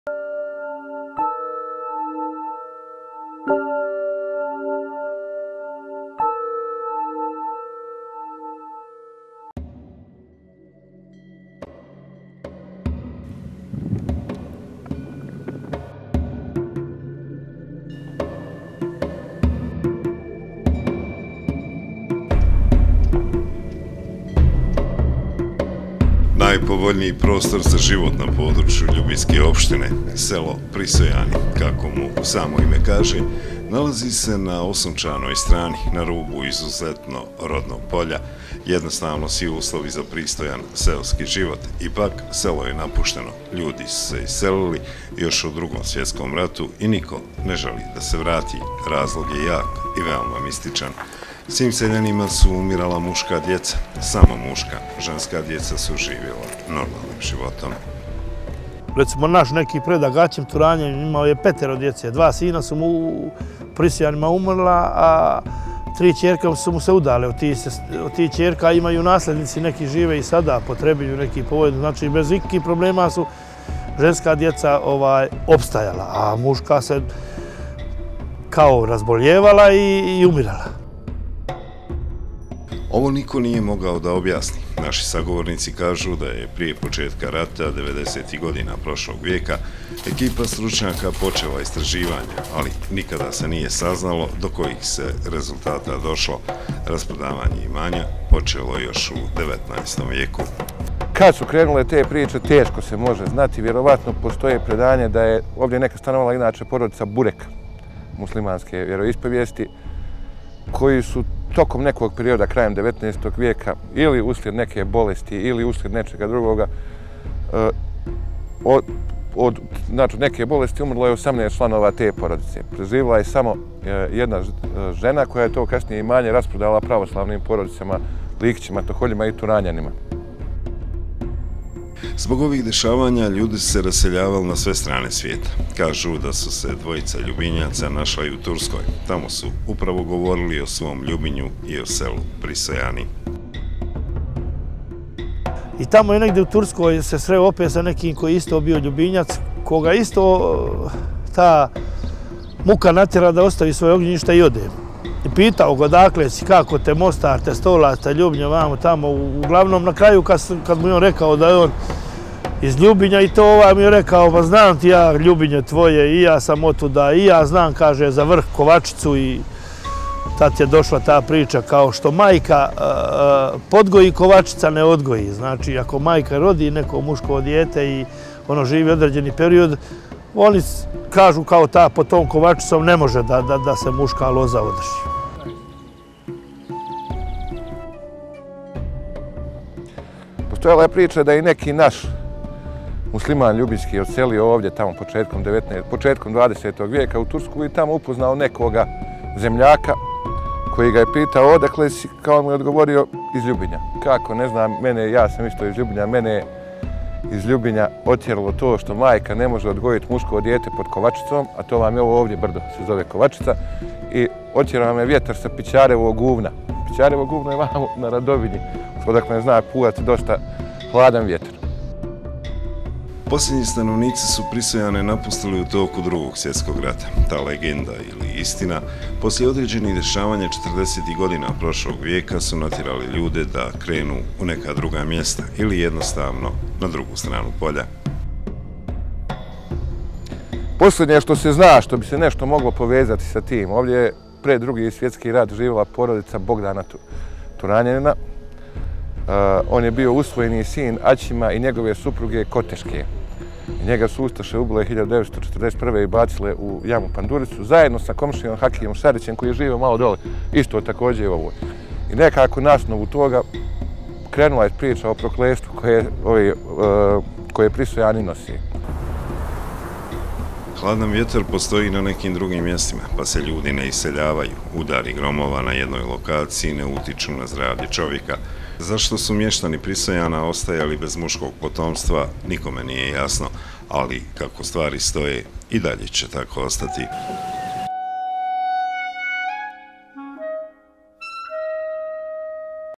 11 06 Selo Prisojani zašto su umirala samo muška djeca
Ljubinjsko selo Prisojani u sebi krije veliku tajnu. Muška djeca su umirla, a da nikome nije jasno zbog čega. U Drugom svjetskom ratu se i posljednji stanovnik iselio,tako da su Prisojani danas prazni.